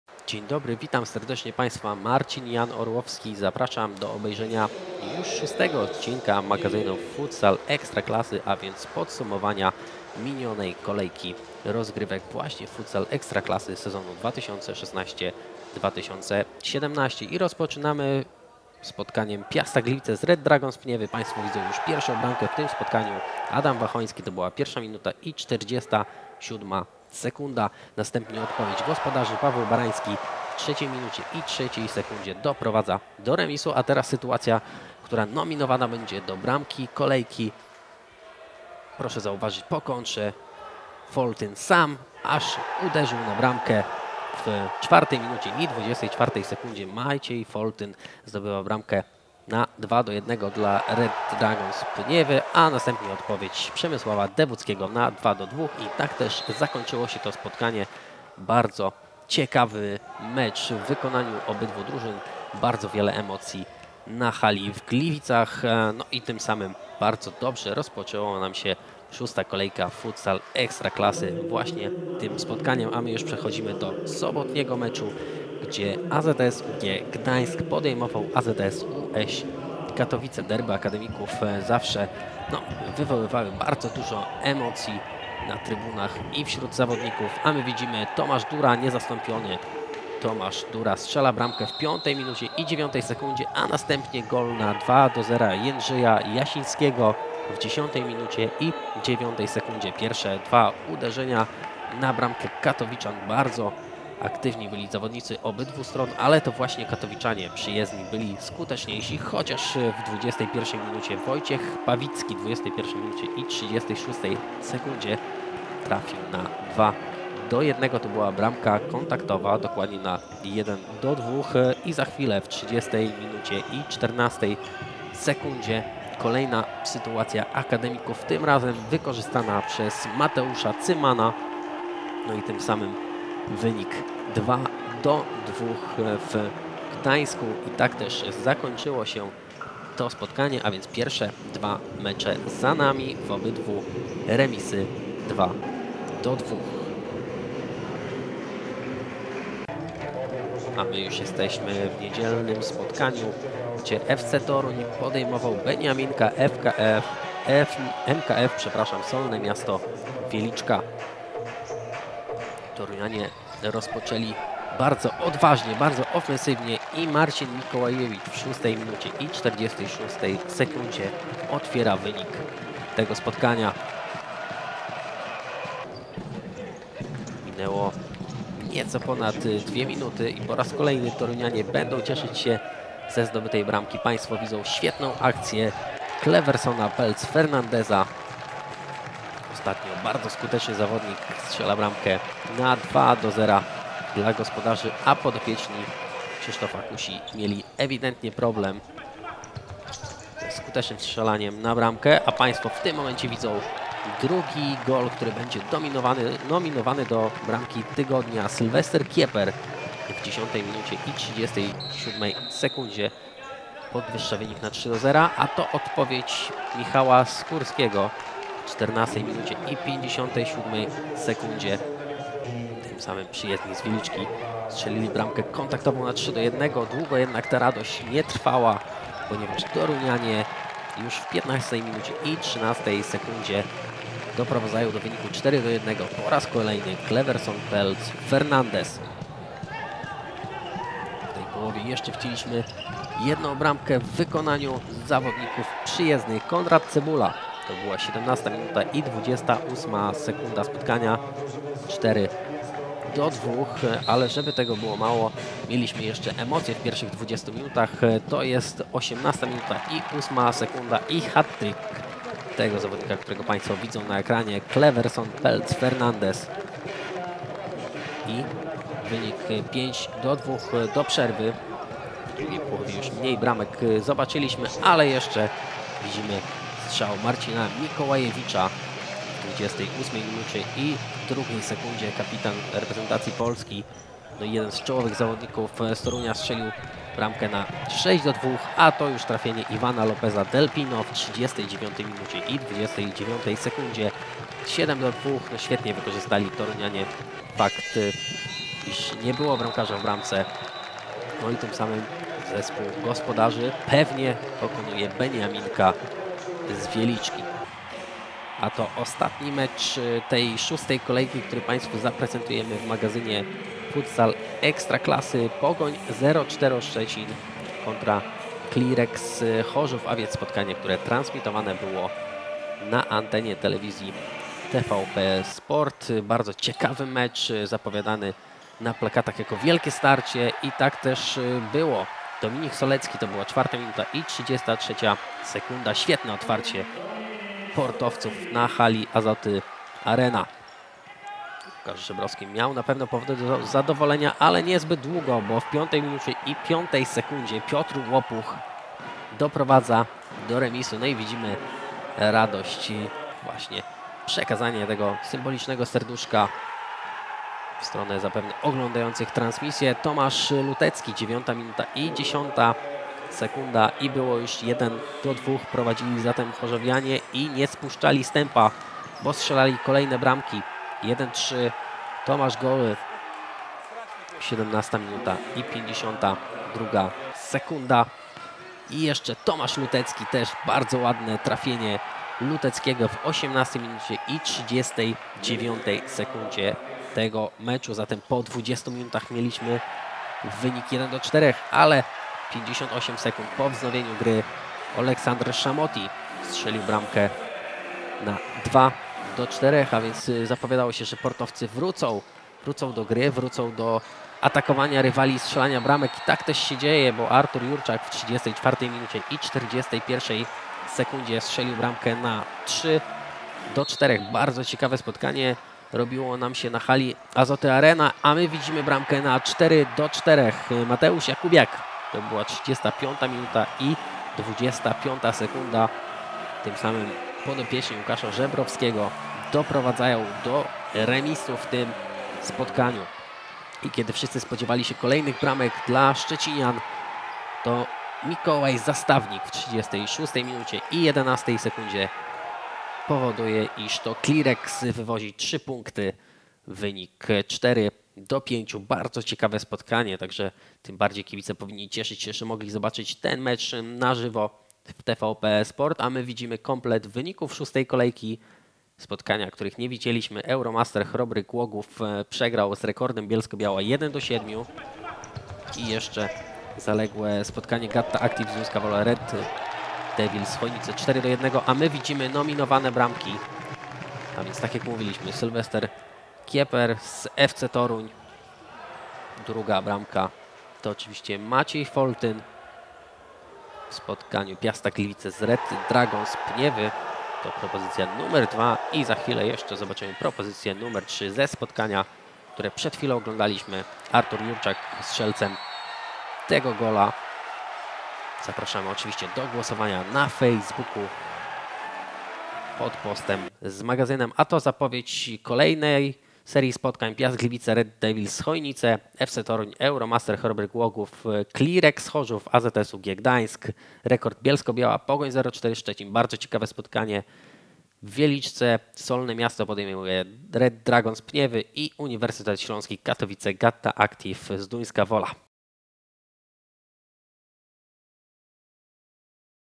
Magazyn STATSCORE Futsal Ekstraklasy / 6. KOLEJKA / 2016/17
Podsumowanie 6. kolejki STATSCORE Futsal Ekstraklasy sezonu 2016/17.